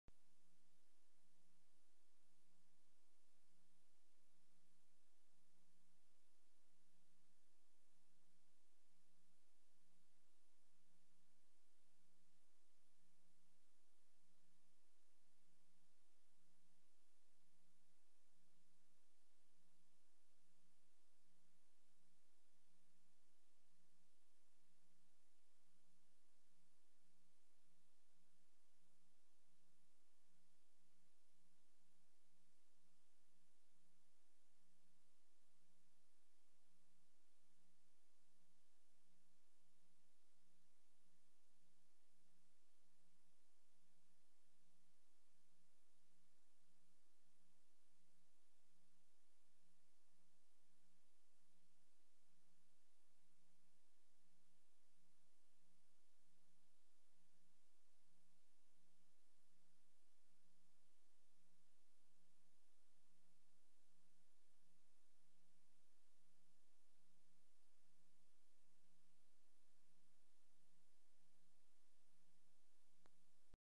BANANAS - NO SOUND
GV Banana Plantation from the air. Various scenes of the harvest. Cutting the stems. Dockside scenes. Bunches being placed in Plastic bags. Ship in harbour for export.